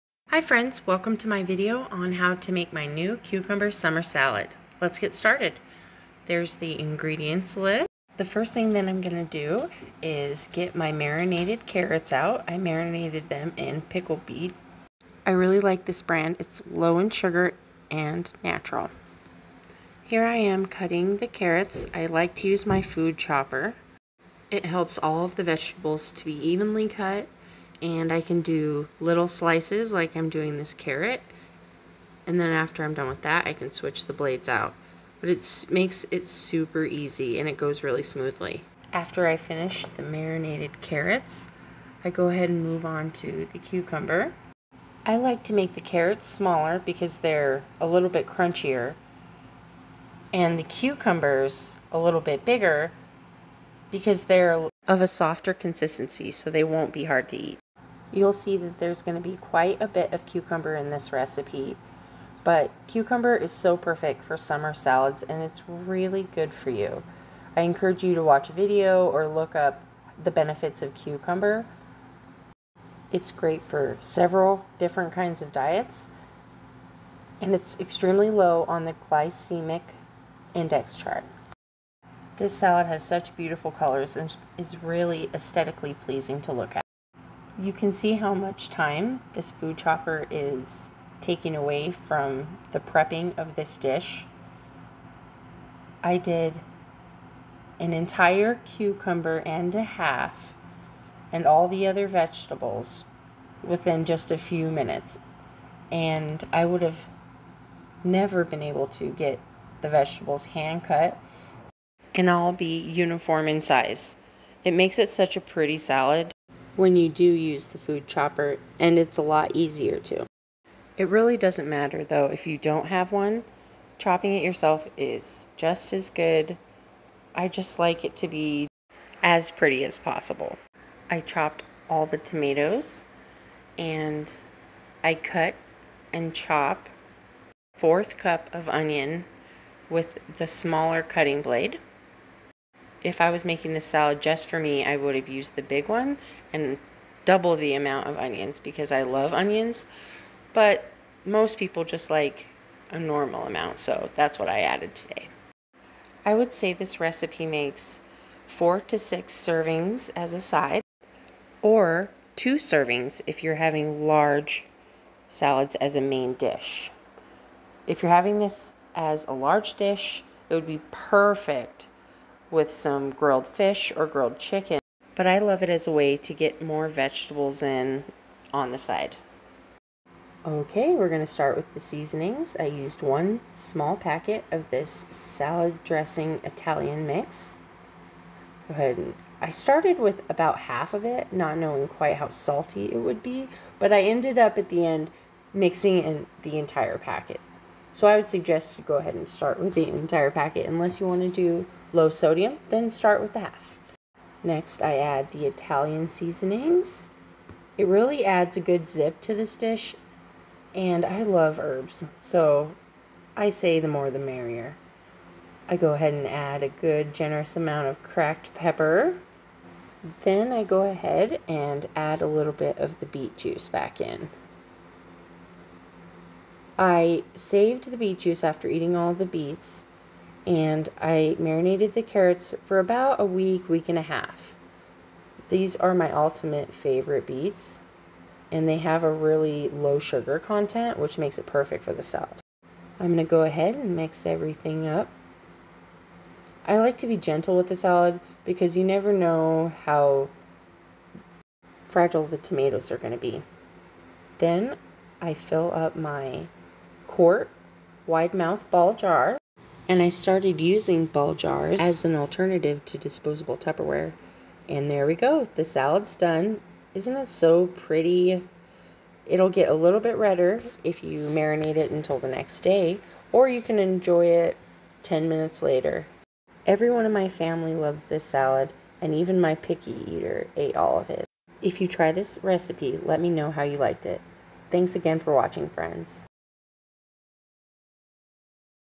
How to make my new cucumber salad
This is a how-to video on making you my new cucumber salad. There are so many amazing benefits to eating cucumber and its perfect  for low carb and diabetic diet. this is a perfect dish to bring to a summer barbecue and it's a great addition to any meal. Try this dish, and let me know what you think!🙂